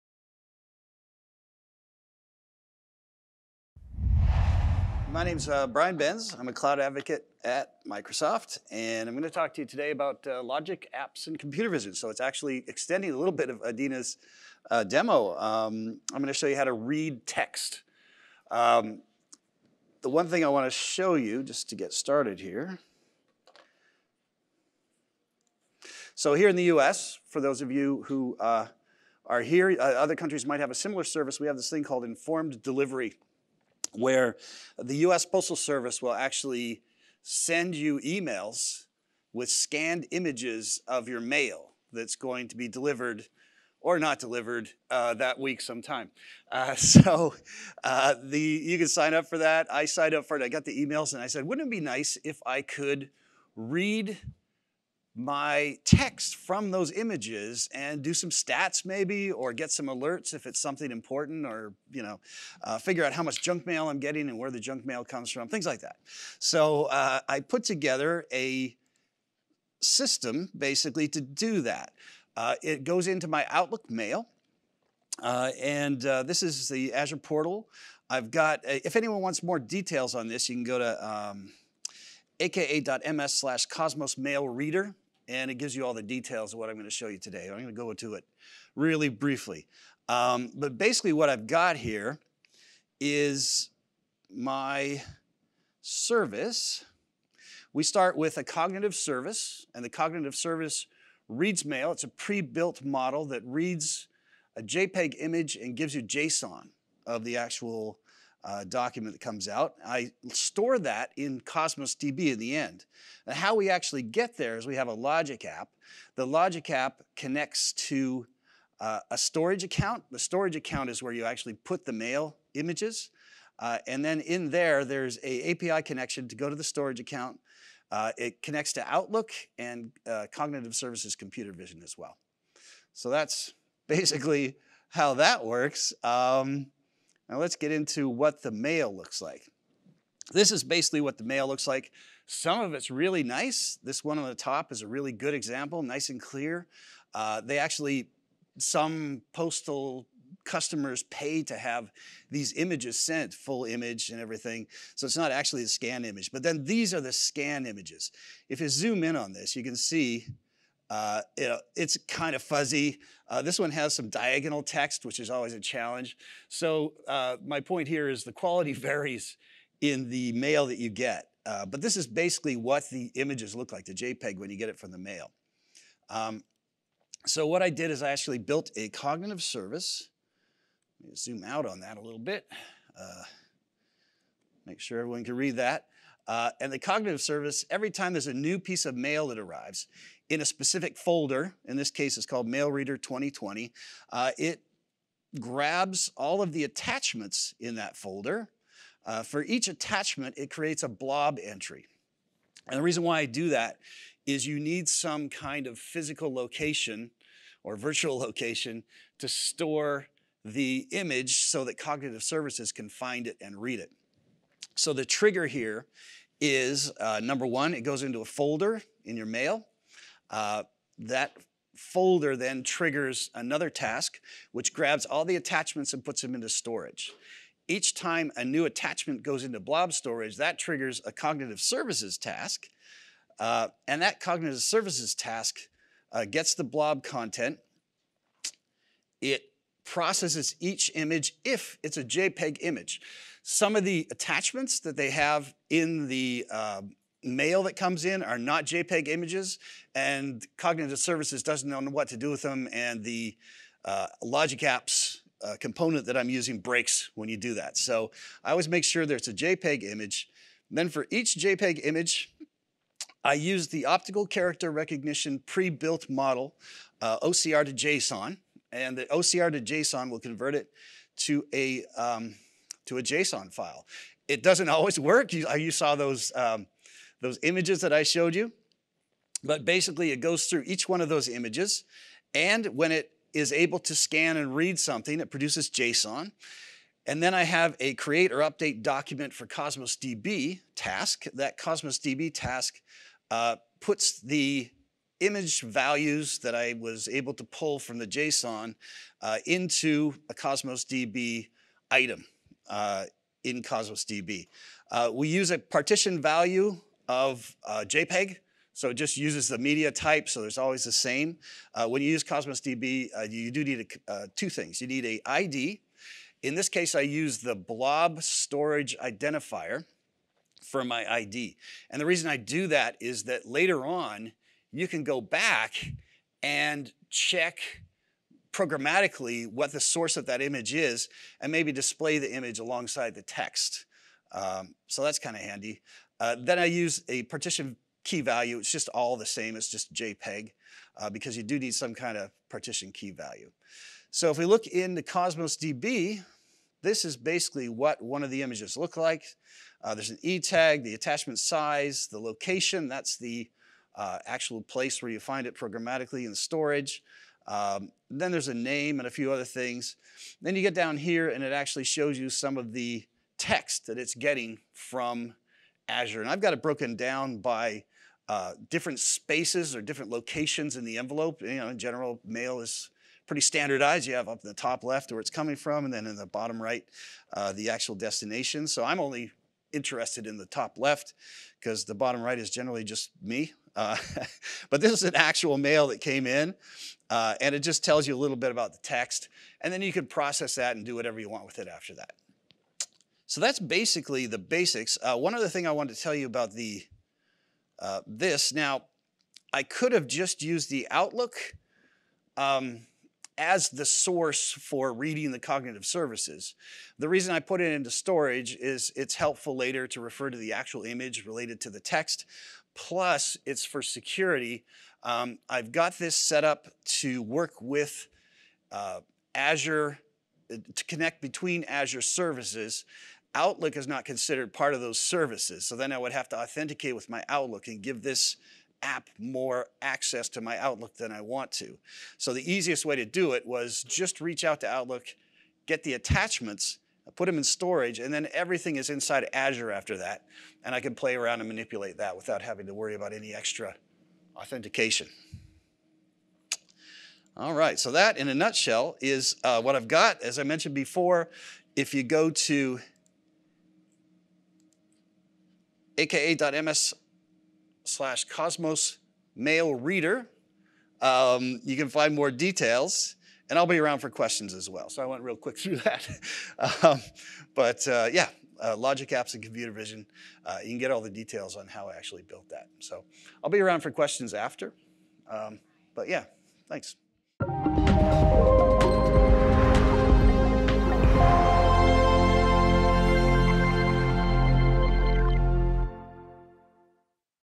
Use Azure Cosmos DB, Azure Logic Apps, and AI to store and analyze images
Learn how to build a solution that collects, stores, and analyzes data from images using Azure Logic Apps, Azure Cosmos DB, Azure Storage, and Azure AI. We walk through an example that extracts address information from images of physical mail that the US Postal System delivers and explore how to analyze and track the data for insights.

This live demo session was delivered as a part of Microsoft Build in Seattle in May 2023.